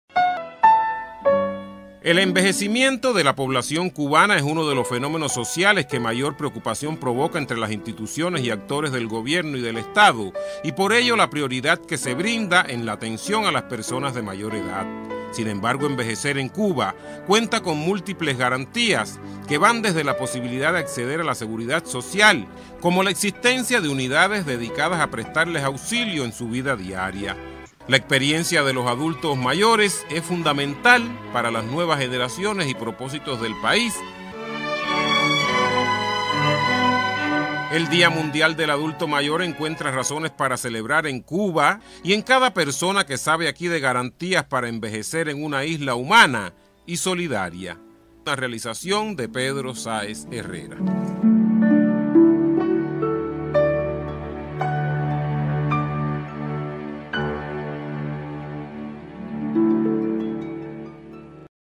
Día Internacional del adulto mayor en Cuba
CON EL objetivo  de promover  políticas y programas  públicos  dirigidos    a las personas de la tercera edad  para  garantizarle una mejor calidad de vida, cada PRIMERO de octubre se celebra el Día internacional  del adulto mayor, efeméride  instituida por la Organización  de las Naciones Unidas desde el año MIL 991.